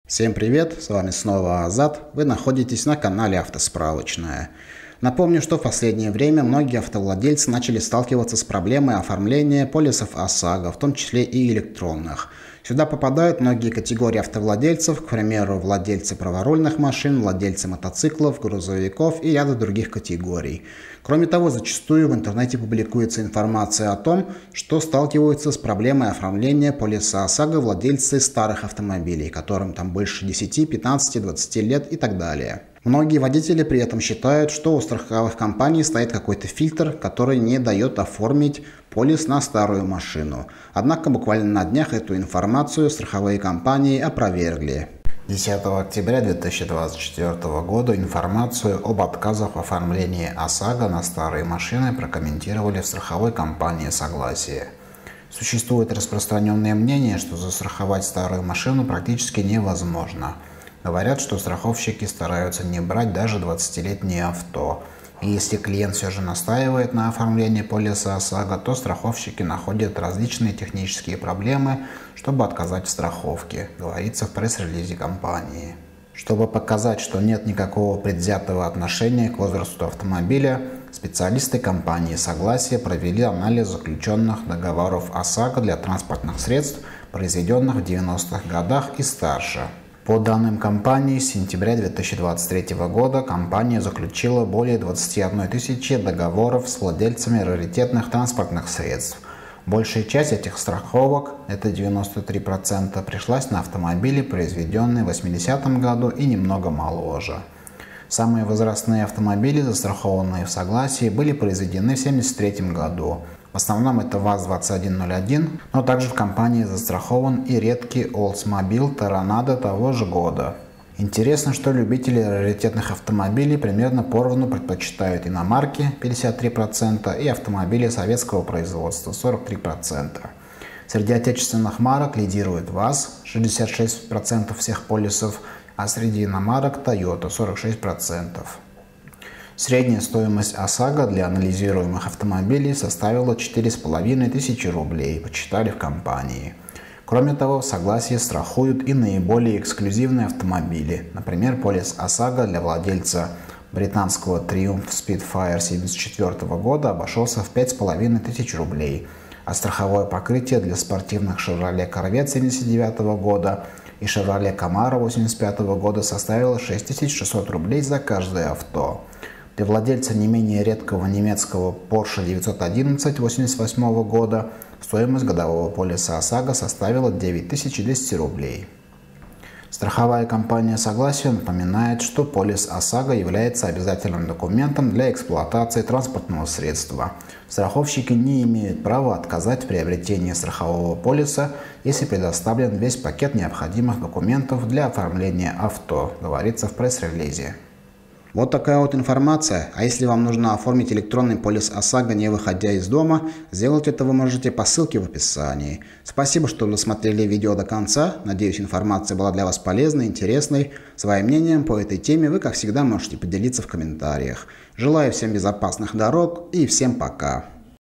ОСАГО на старые машины: последние новости 2024
Многие водители жалуются, что в 2024 году не могут оформить ОСАГО на старые автомобили